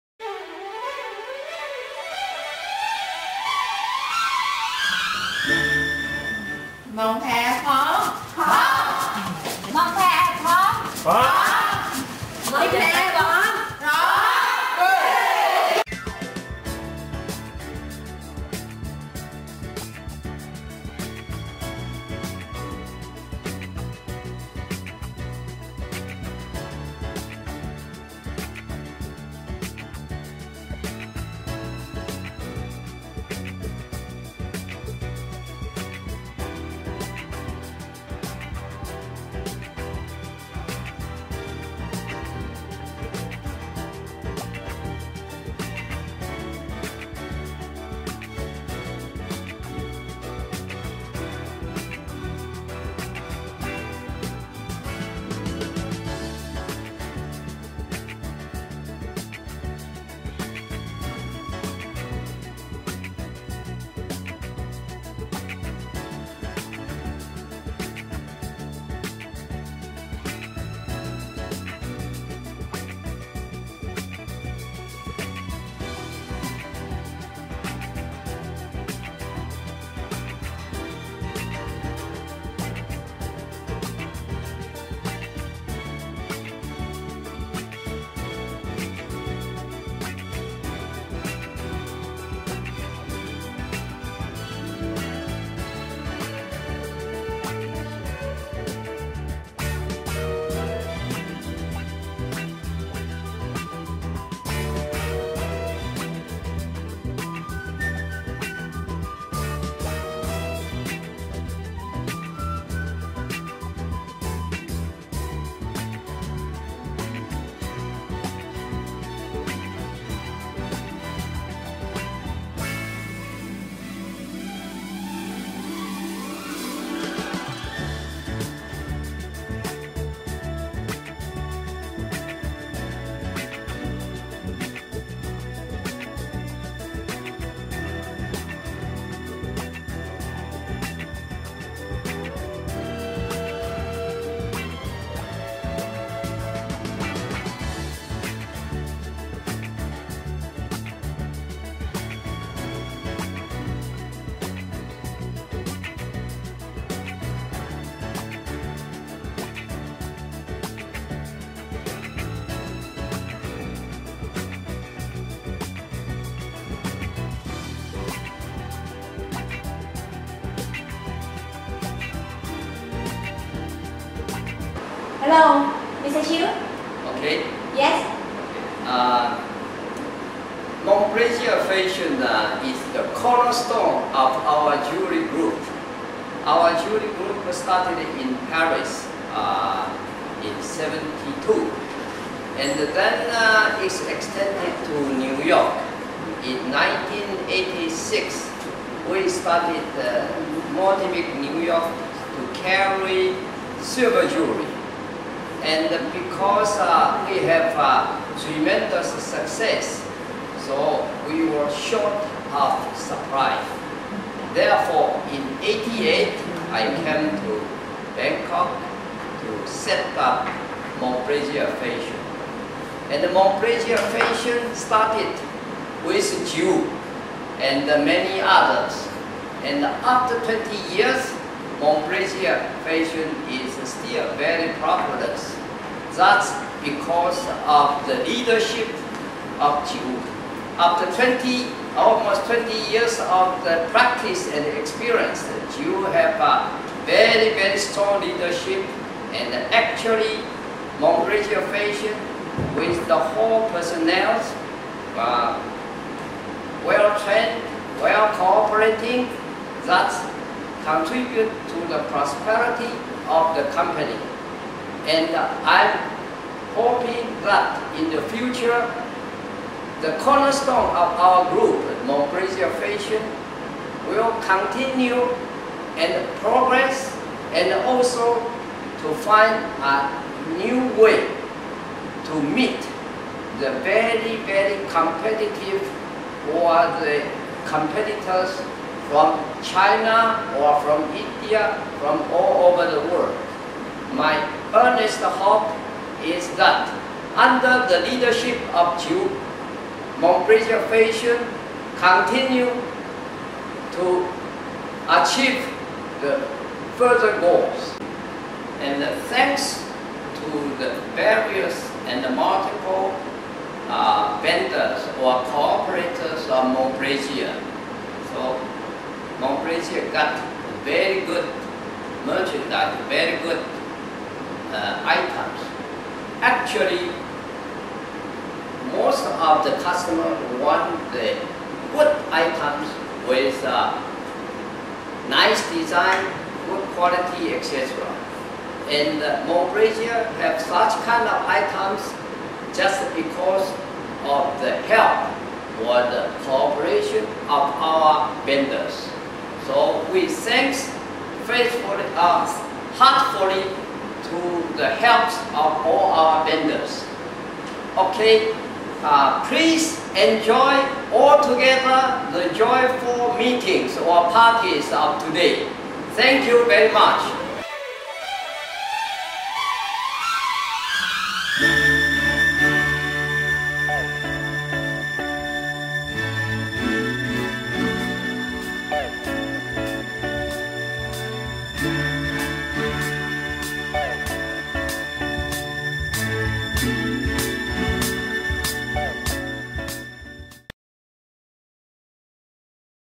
20th Anniversary of Monplaisir Fashion - No.1 Marcasite Silver Jewelry Thai Manufacturter&Exporter
Monplaisir Fashion Company is Leading Marcasite Silver Jewelry Thailand Manufacturer and Exporter including Marcasite Silver with Agate stones,Natural shell,Pearl,CZ and Resin Enamel.